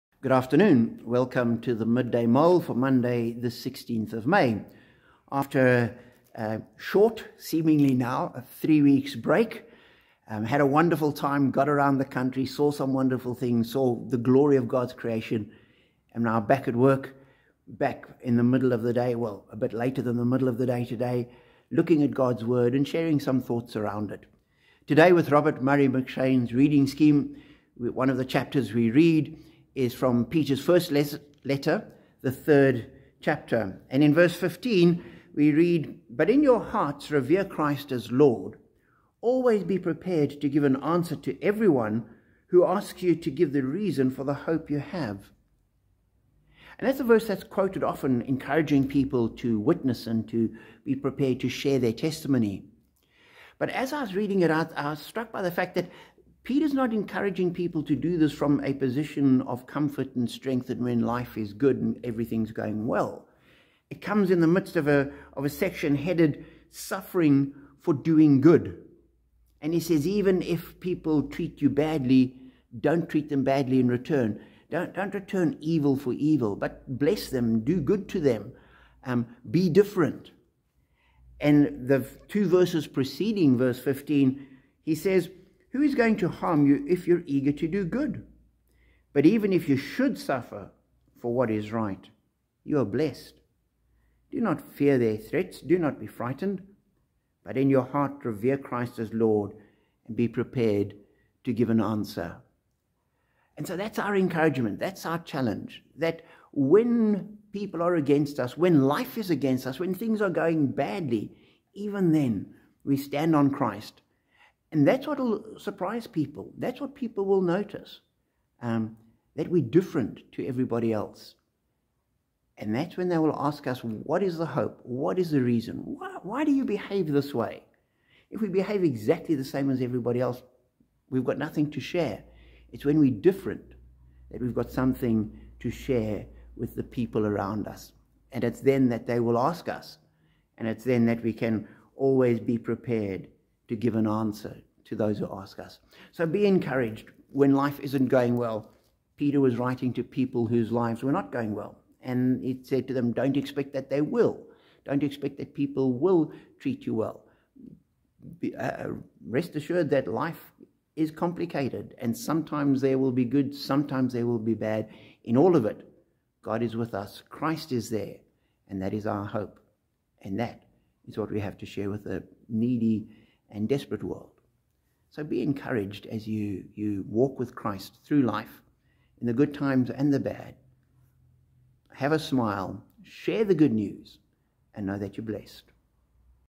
Midday Mull 22 05 16
We should always share the reason why we have hope – even in the face of suffering and especially when treated badly. (1 Peter 3:15)